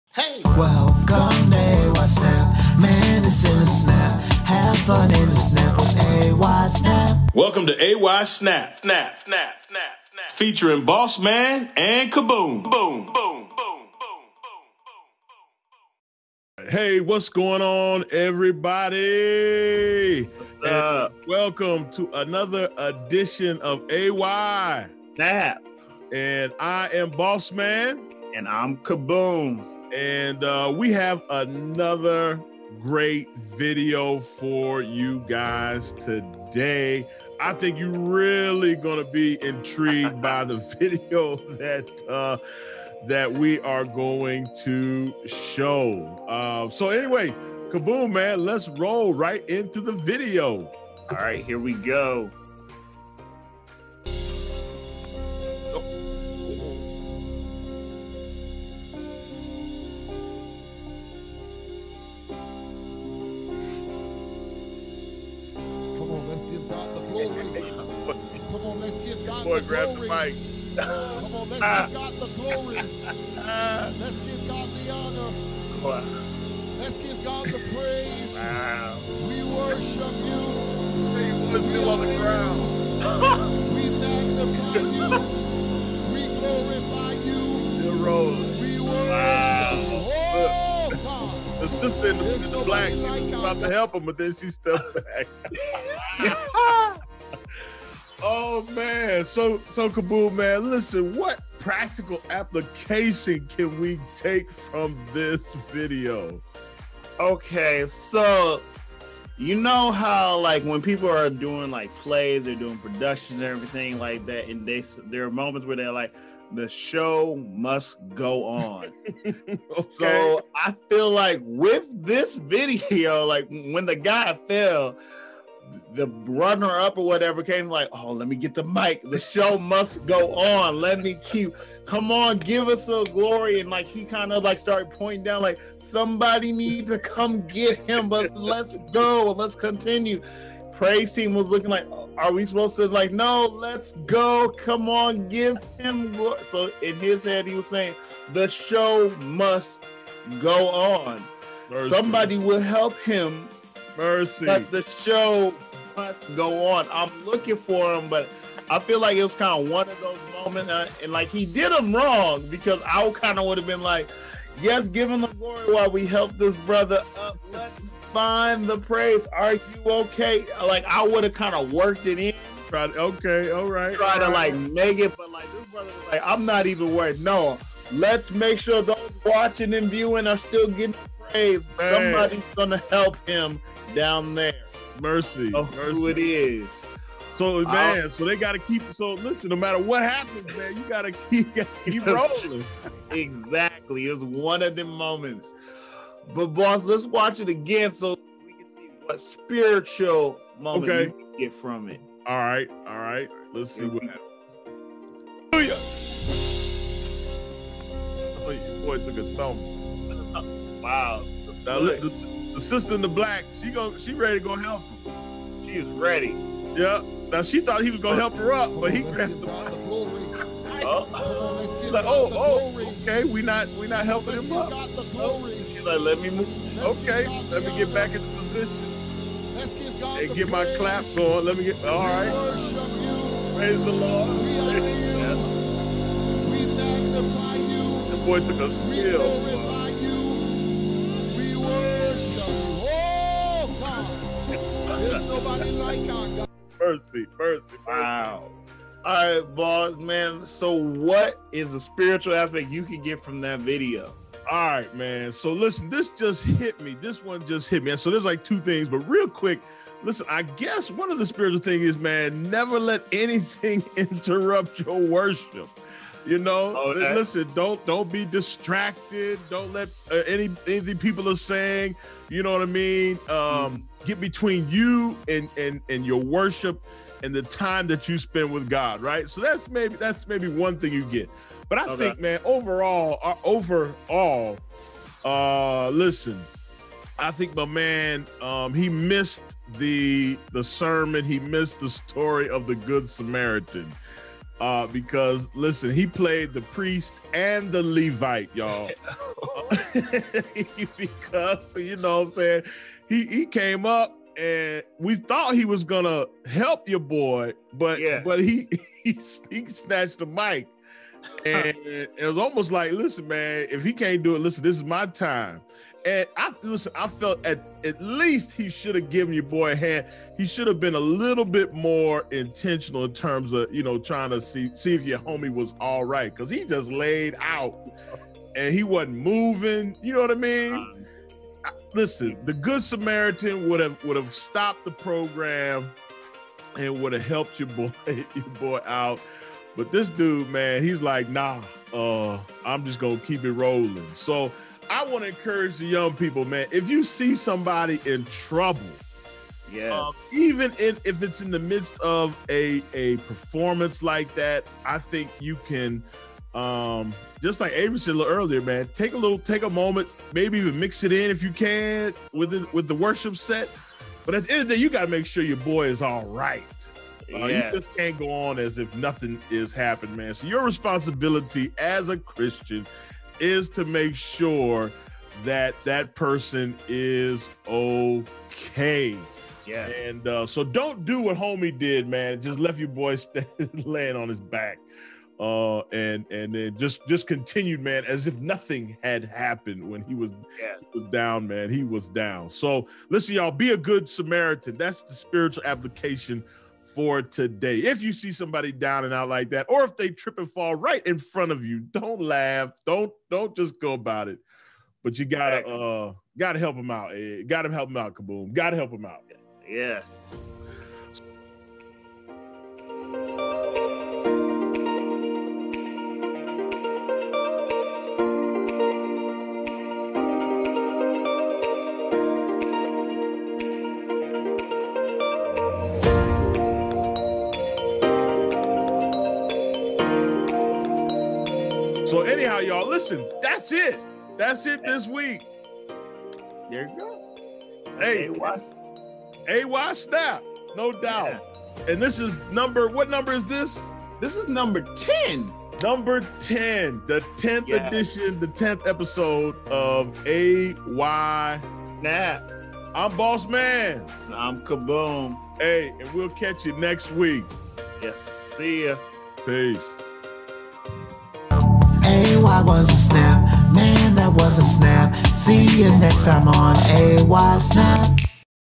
AY Snap - Ep. 10 "The Show Must Go On or NAH"
That's right AY Snap is back again! Tune in as Bossman & Kaboom discuss how when mistakes happen choices have to made. Don't miss out for we are always done in a SNAP!